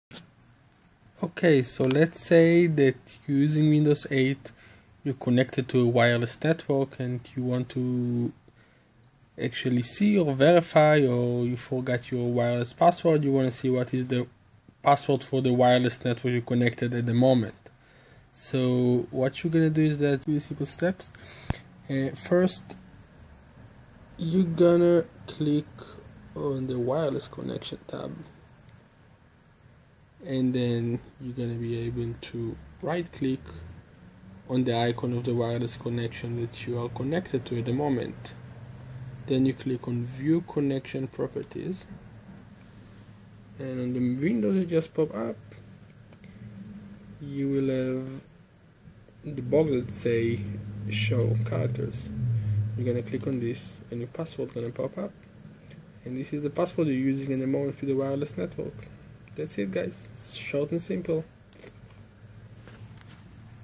How to find your Wireless network Key password on windows 8
This Video will show you how to find or recover your wireless password key of your existing network while using windows 8 or 8.1